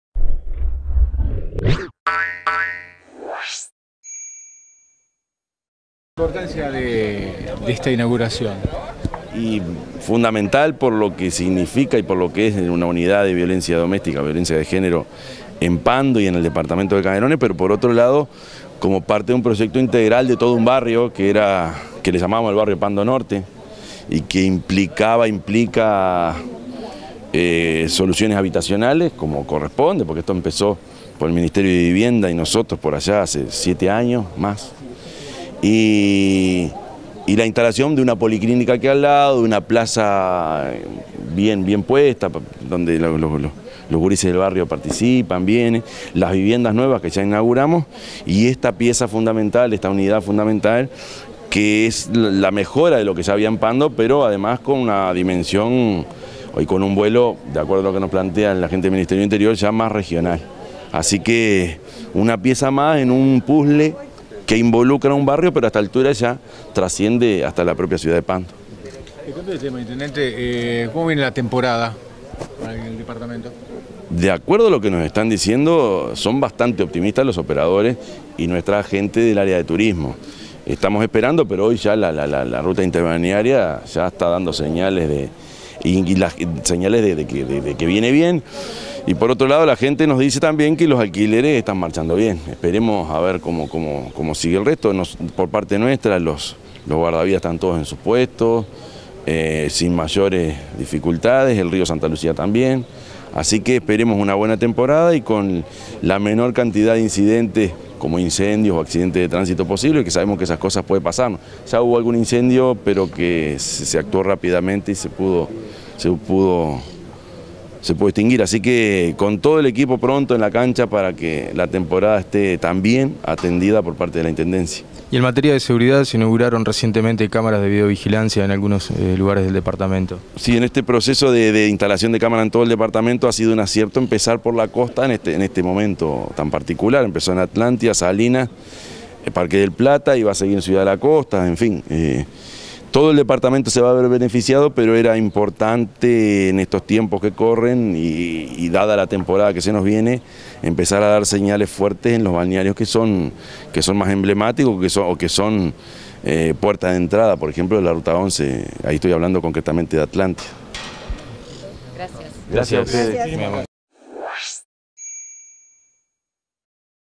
+ Rueda de Prensa Intendente Orsi en Pando 20/12
Se realizó la inauguración de la nueva infraestructura de la Unidad Especializada de Violencia Doméstica y Género de la ciudad de Pando.
En ese marco el intendente de Canelones, Prof. Yamandú Orsi charló con los medios de comunicación.